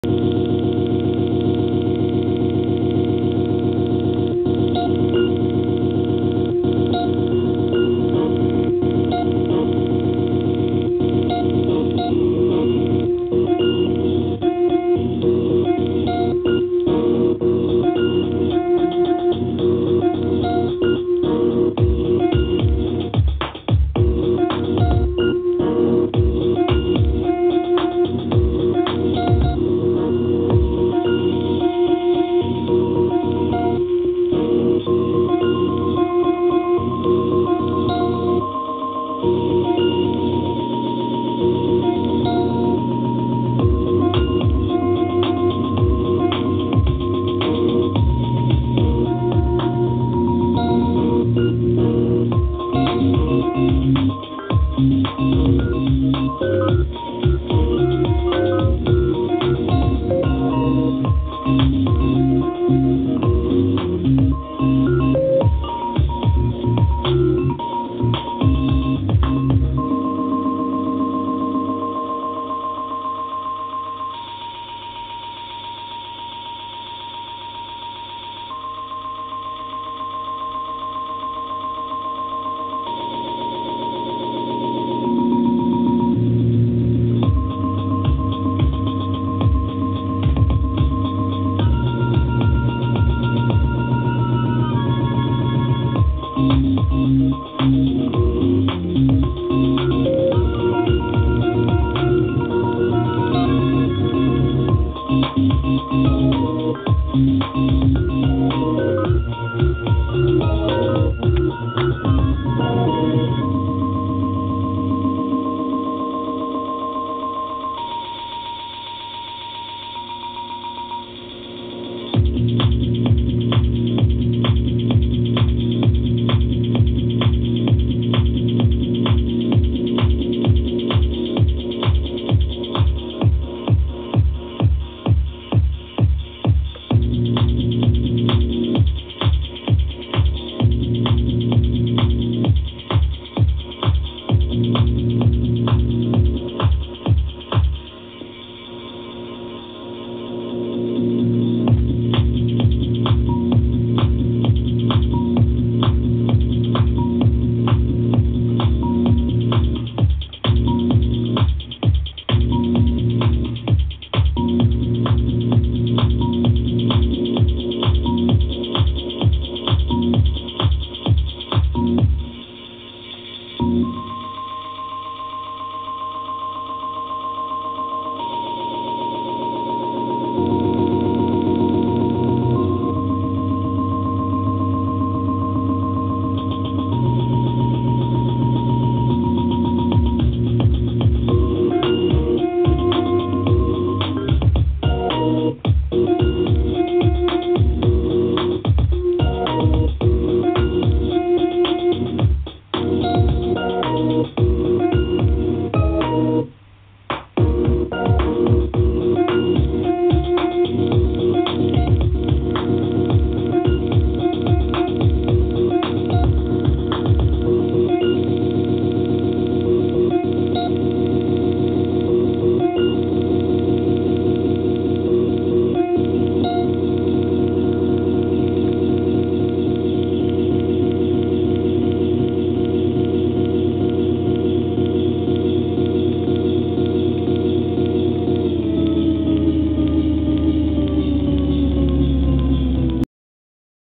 Alexis MPC 1000 16 Track Mute-Play
playing a new song on the mpc using Track-Mute function.